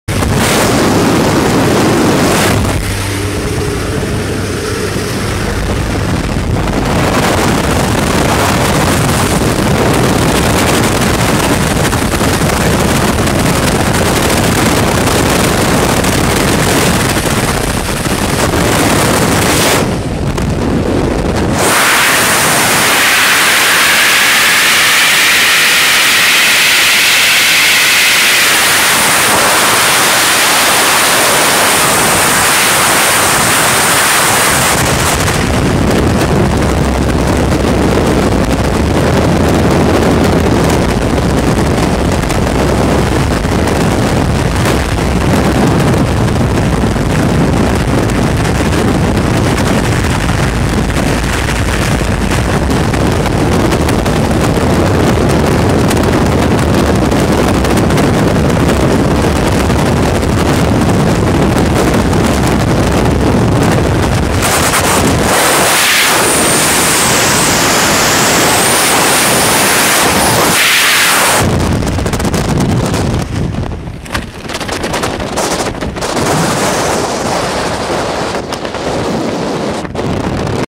Hoop Dive
At the first-ever "Flippin' Fun" event, organized by Jim McCormick, we did this classic "hoop dive", during which each jumper attempts to dive through a hula hoop. We (and the other team as well) broke the hoop on the first try. Lots of fun!